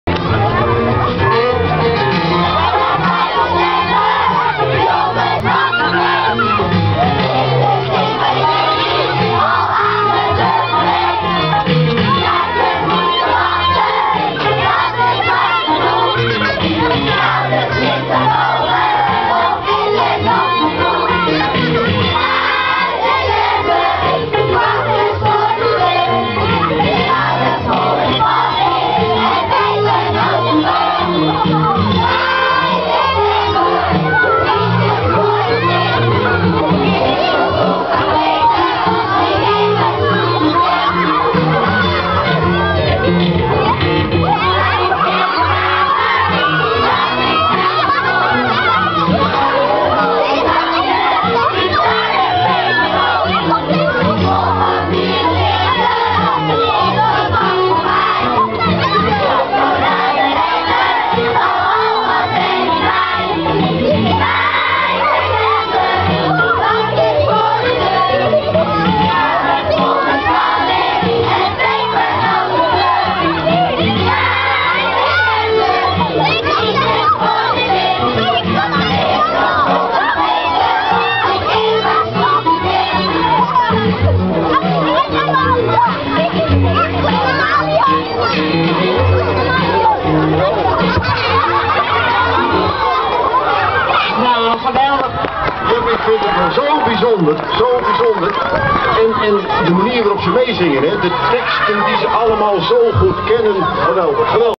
Sint op het Lichtschip 2008
Groep 5 en 6 zingen een lied voor de Sint